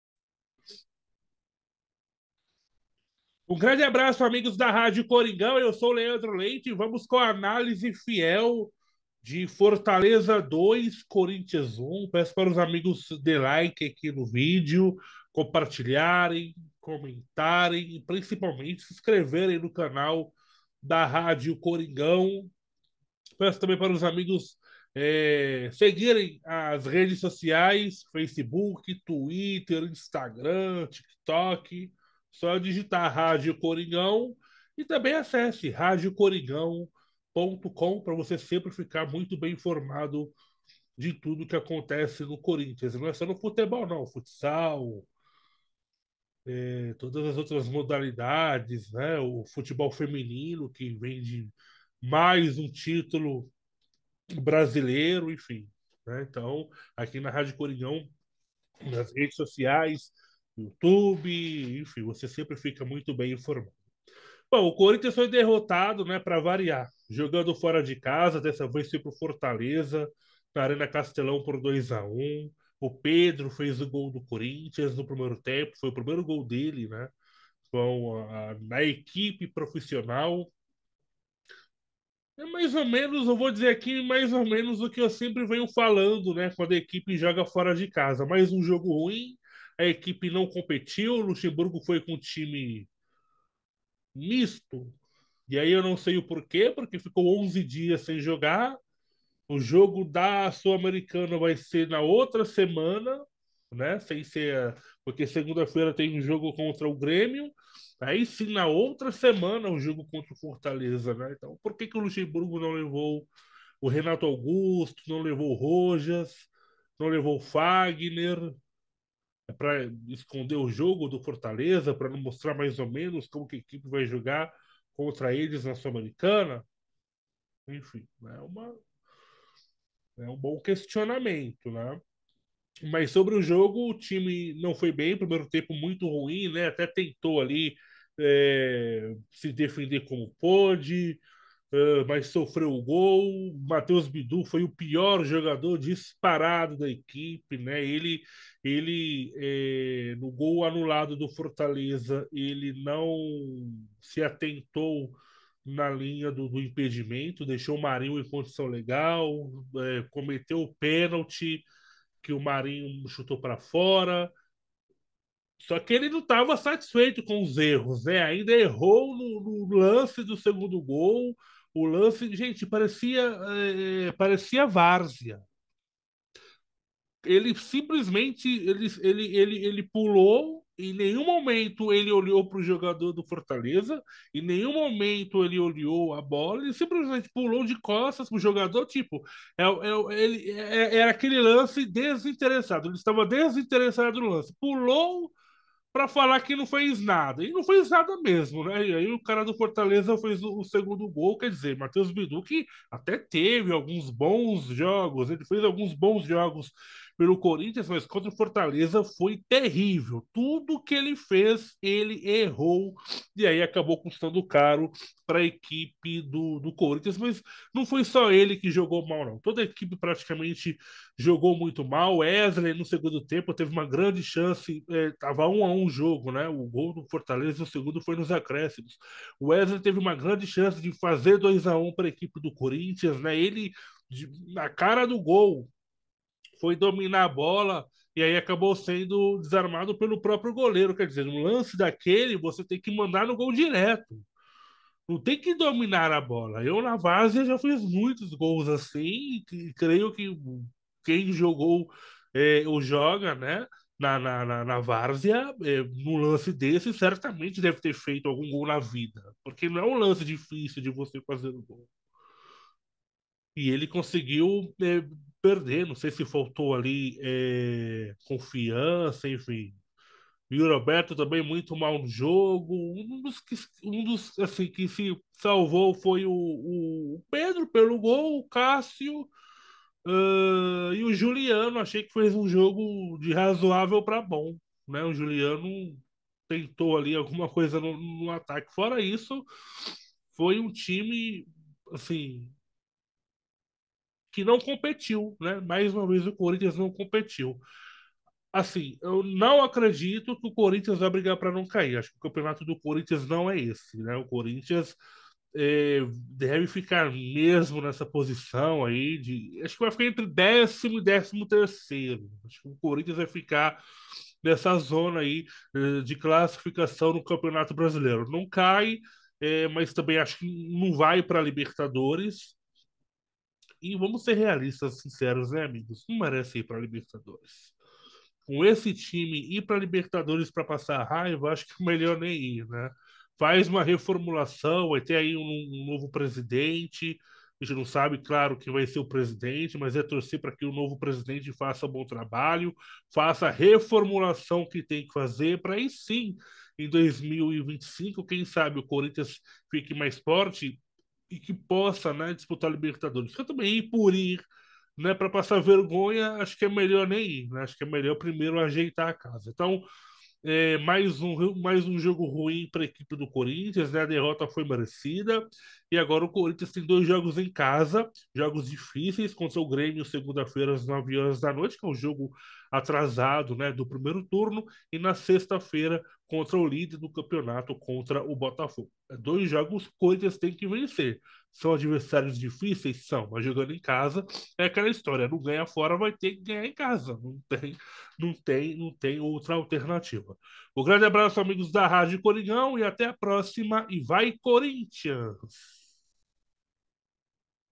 Luxa na corda bamba - Análise Fiel
Leandro Leite analisa a derrota do Corinthians no Brasileirão para o Fortaleza